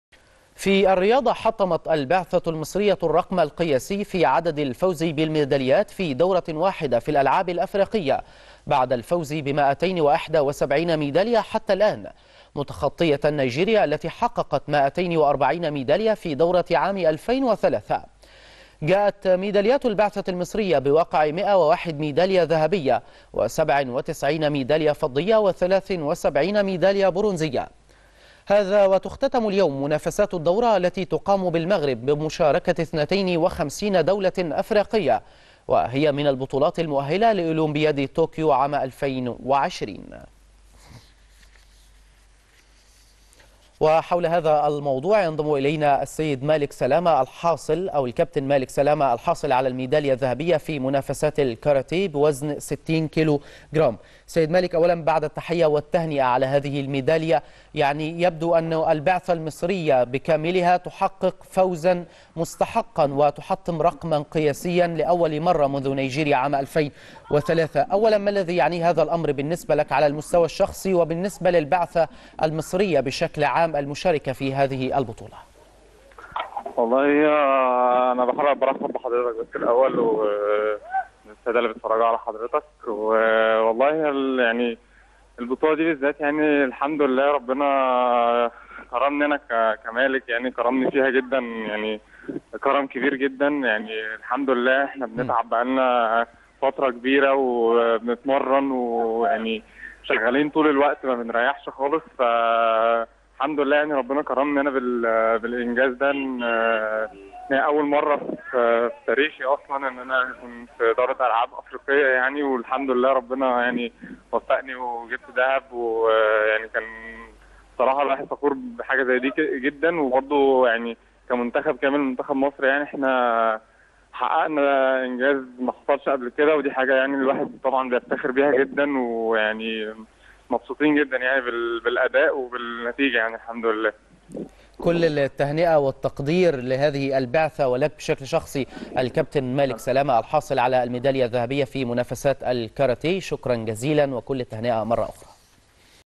كابتن مالك سلامة: جهزت للبطولة منذ فترة كبيرة وتكلل ذلك بالذهبية
قال مالك سلامة، الحاصل على الميدالية الذهبية في منافسات الكاراتيه بدورة الألعاب الإفريقية، في مداخلة هاتفية بنشرة أخبار_TeN، جهزت للبطولة منذ فترة كبيرة وتكلل ذلك بالذهبية.